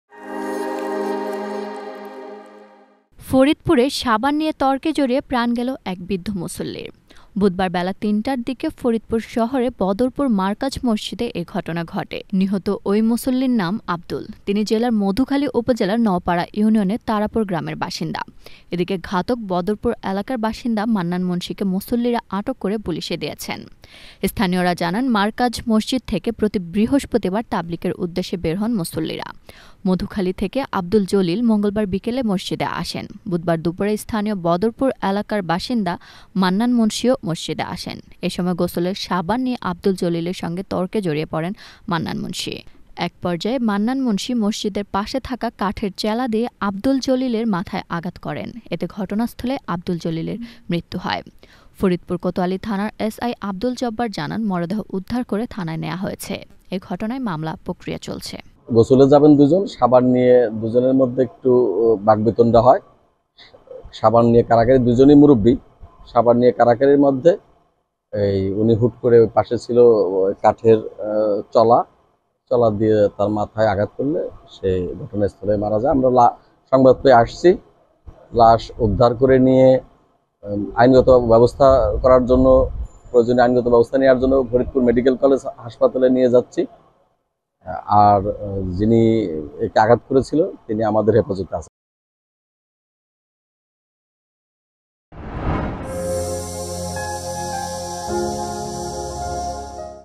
ফরিদপুরে সাবান নিয়ে তর্কে জড়িয়ে প্রাণ গেলো এক বৃদ্ধ মুসল্লীর | Channel S News | Channel S 2023
ফরিদপুরে সাবান নিয়ে তর্কে জড়িয়ে প্রাণ গেলো এক বৃদ্ধ মুসল্লীর। বুধবার বেলা ৩টার দিকে ফরিদপুর শহরের বদরপুর মারকায মসজিদে এ ঘটনা ঘটে। নিহত ওই মুসল্লীর নাম আব্দুল। তিনি জেলার মধুখালী উপজেলার নওপাড়া ইউনিয়নের তারাপুর গ্রামের বাসিন্দা...

1: News :
2: Entertainment News :
3: Sports News :